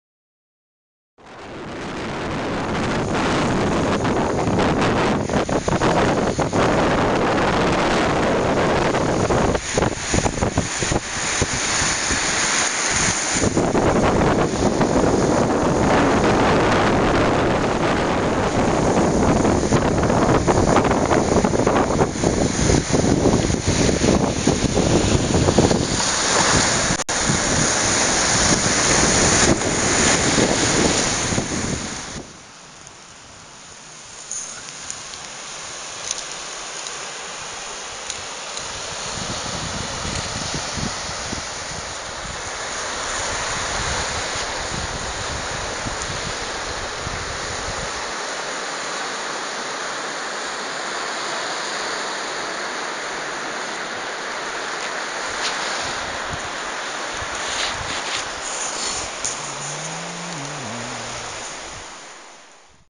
Kolka Cape (Kolkas rags) 31.12.2013
Day spent hiking on the coastline and in the forests of Cape Kolka, Latvia. This NewYear will be without the snow and winter feeling, so let it be a bit different...